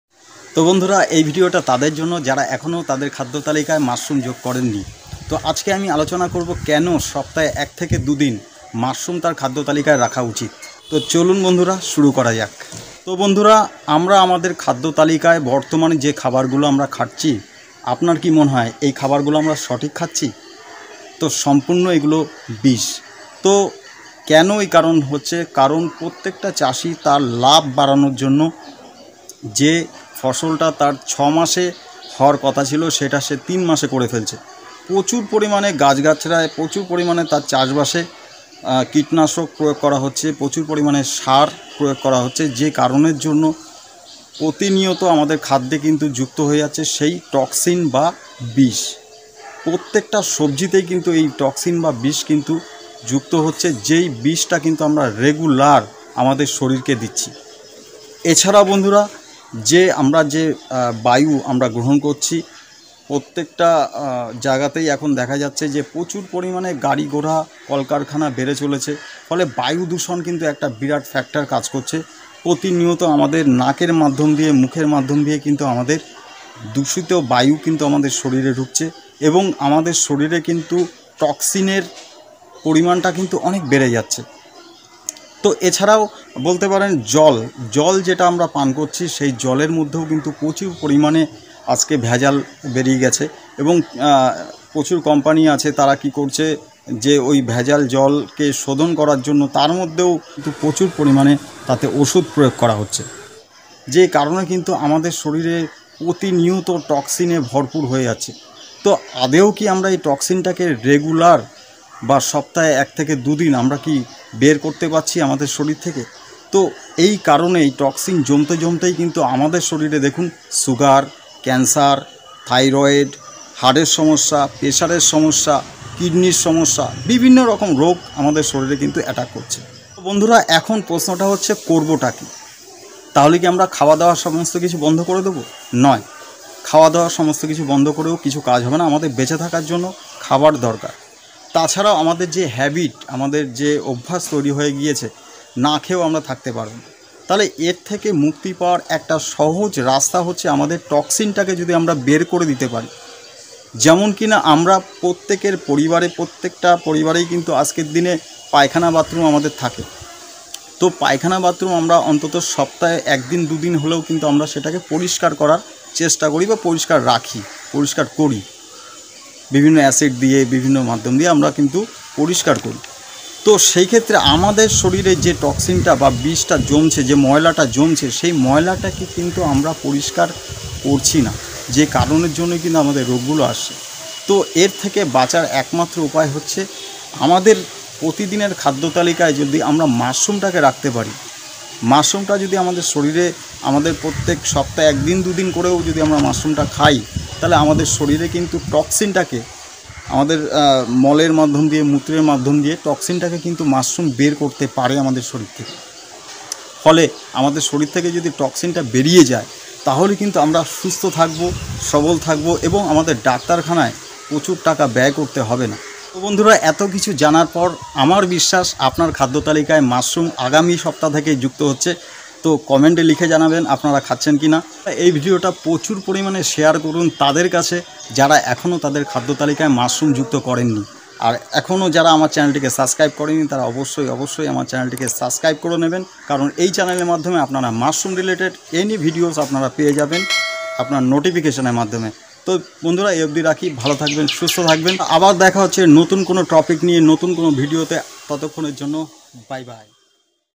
মাশরুম কেন সপ্তাহে ১-২ দিন খাওয়া উচিৎ?।। Why Eat the Mushroom weekly 1-2 days
বন্ধু আজকের ভিডিও তে আমি আলোচনা করবো কেন আমাদের মাশরুম সপ্তাহে ১-২ দিন খাওয়া উচিৎ। আশাকরি ভিডিও টি আপনার ভালো লাগবে।